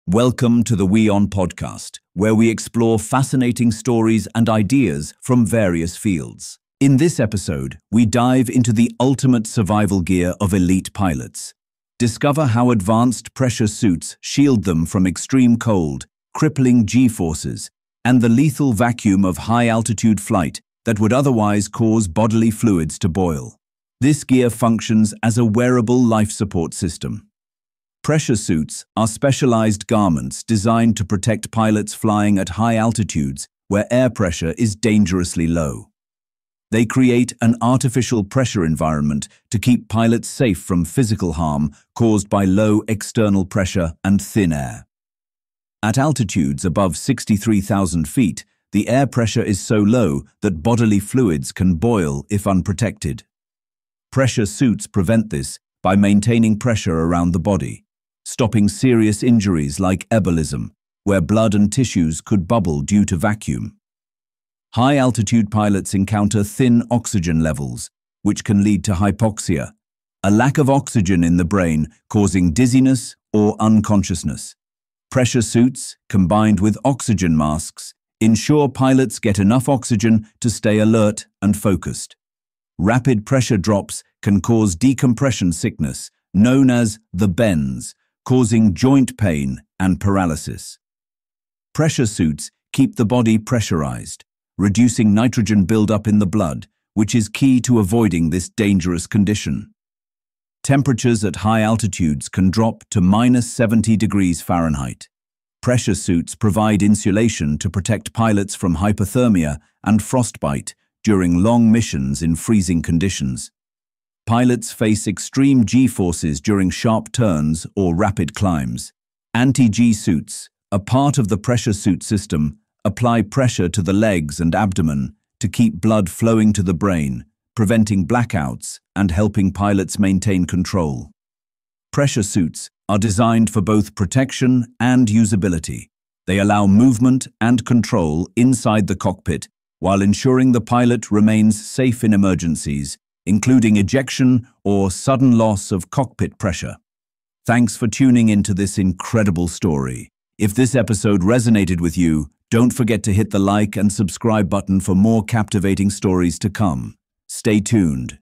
Why Fighter Pilots Wear Pressure Suits to Survive High Altitude Flight | WION Podcast
Dive into the engineering marvel of the pressure suit, the ultimate survival gear for elite pilots. This episode uncovers how this specialised garment protects aviators from the lethal vacuum of high altitude, where unprotected bodily fluids could boil at 63,000 feet. We detail the suit's critical roles: providing life-sustaining oxygen, preventing decompression sickness, insulating against extreme cold, and functioning as an anti-G system to fight deadly G-forces during intense maneuvers.